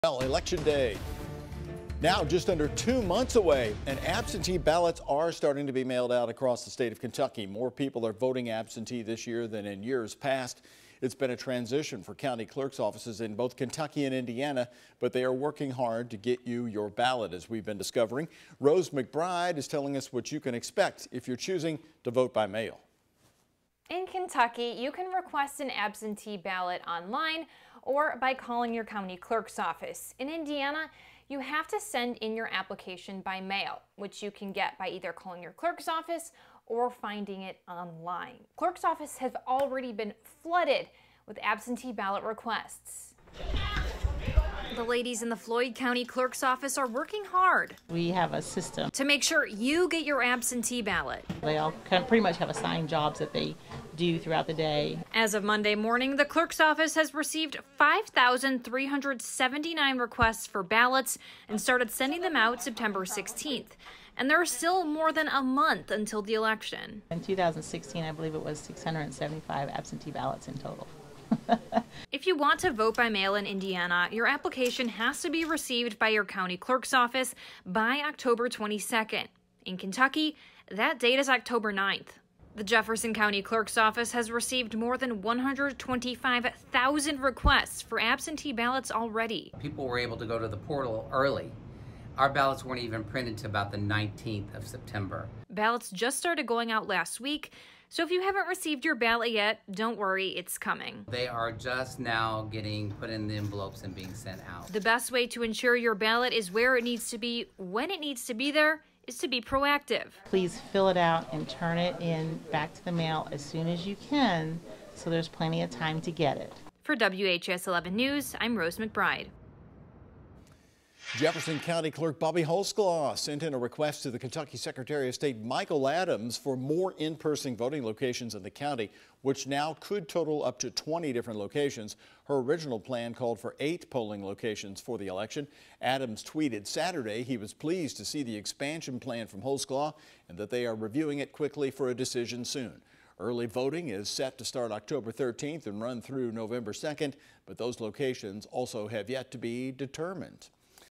What you need to know about absentee voting in Kentucky and Indiana
Many of Kentuckians and Hoosiers have been worried about voting by mail. Here's what you need to know to vote absentee and tracking your ballot.